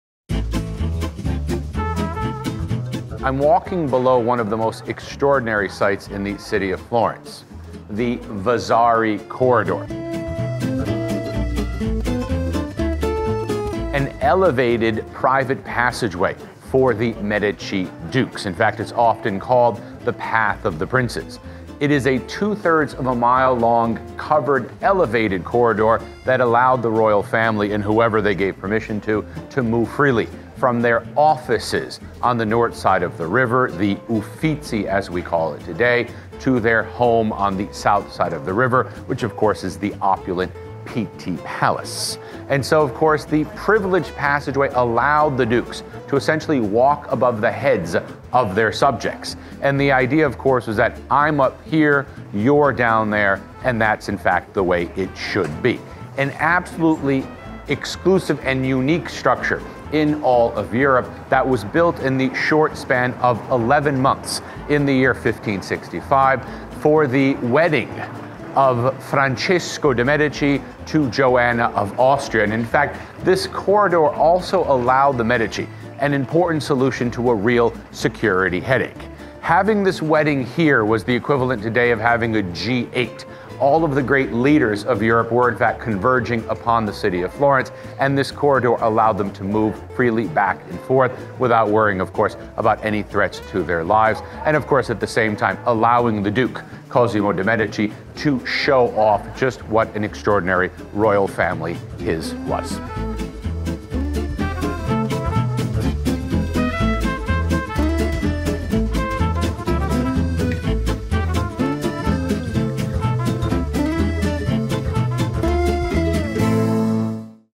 ROCKY'S ITALY: Florence - The Vasari Corridor
Discover one of the world's most exclusive passageways! Nearly 2/3 of a mile long, the Vasari Corridor allowed royal members of the Medici family in Florence, Italy, to move safely and securely between their home and administrative hub above the heads of their subjects.

3. ROCKY'S PODCAST | "REBUILDING THE RENAISSANCE" [available in all major podcast platforms]